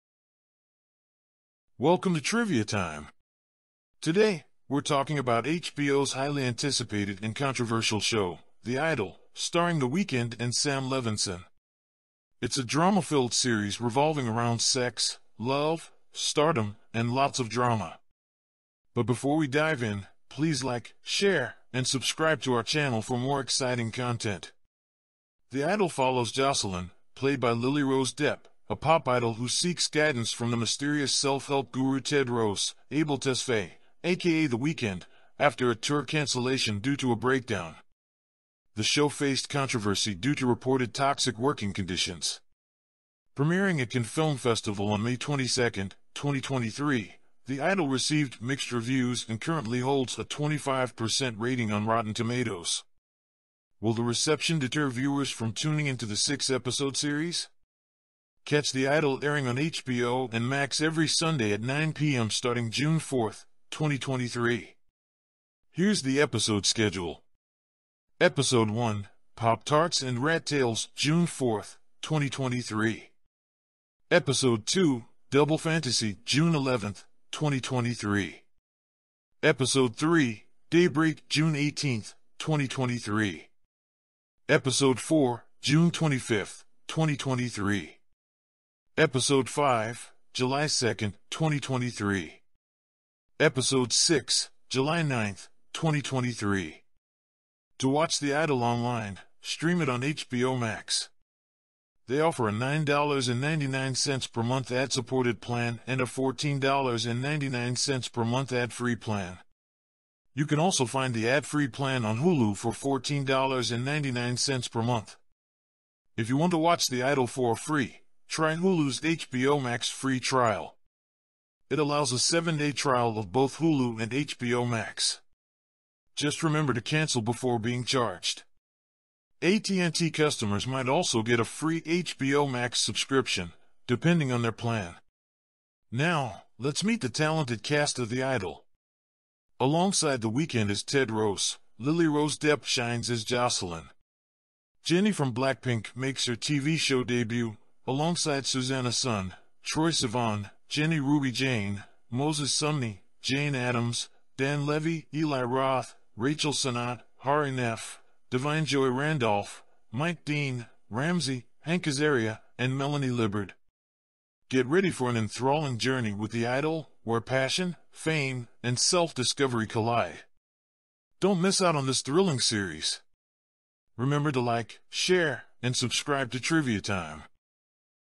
Here’s How To Watch The Idol For Free—The Weeknd’s New Controversial Show
Welcome to Trivia Time! Today, we're talking about HBO's highly-anticipated and controversial show, "The Idol," starring The Weeknd and Sam Levinson. It's a drama-filled series revolving around sex, love, stardom, and lots of drama.

"The Idol" follows Jocelyn (played by Lily-Rose Depp), a pop idol who seeks guidance from the mysterious self-help guru Tedros (Abel Tesfaye, aka The Weeknd) after a tour cancellation due to a breakdown. The show faced controversy due to reported toxic working conditions.

Premiering at Cannes Film Festival on May 22, 2023, "The Idol" received mixed reviews and currently holds a 25% rating on Rotten Tomatoes. Will the reception deter viewers from tuning in to the six-episode series?

Catch "The Idol" airing on HBO and Max every Sunday at 9 p.m. starting June 4, 2023. Here's the episode schedule: